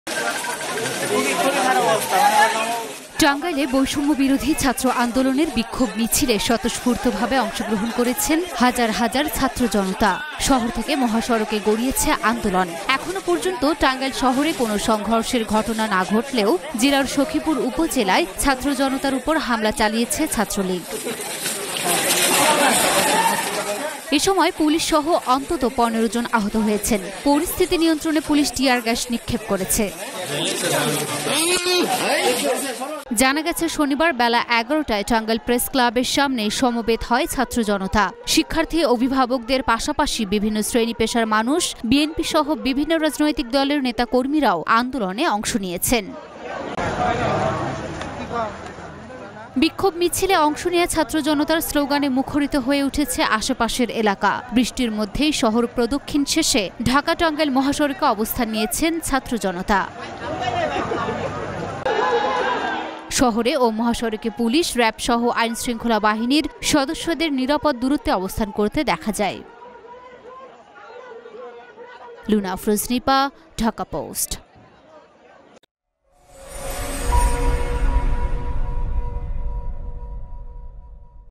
সখীপুরে শিক্ষার্থী‌দের ওপর ছাত্রলীগের হামলা | Tangail | Quota | Quota Issue | Dhaka Post
টাঙ্গাইলে বৈষম্যবিরোধী ছাত্র আন্দোলনের বিক্ষোভ মিছিলে স্বতস্ফূর্তভাবে অংশগ্রহণ করেছেন হাজার হাজার ছাত্রজনতা। শহর থে‌কে মহাসড়‌কে গ‌ড়ি‌য়ে‌ছে আন্দোলন.